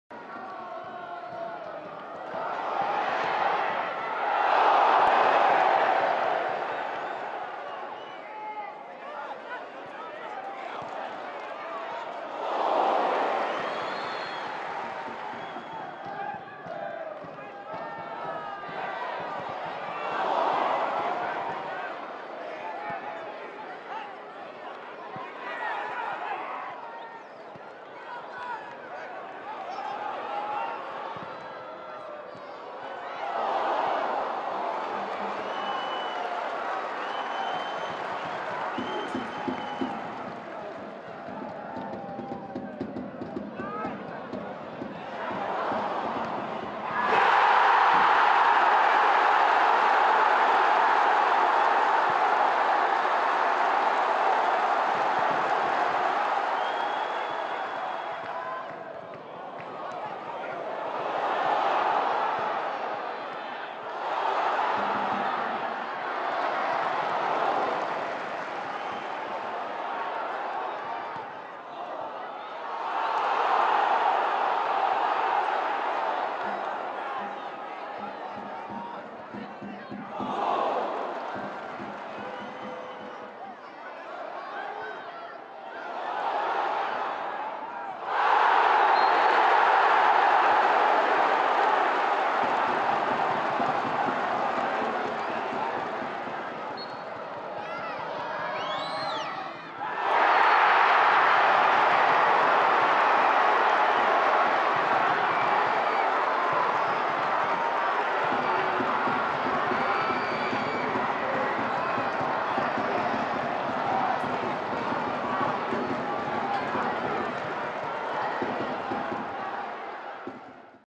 Laing strike decides Lanarkshire Derby | Highlights
Louis Laing scored the deciding goal at New Douglas Park to secure a Lanarkshire Derby win for Motherwell. Accies struggled to clear their lines after a corner from the visitors. Laing capitalised on the scramble and rifled the ball home.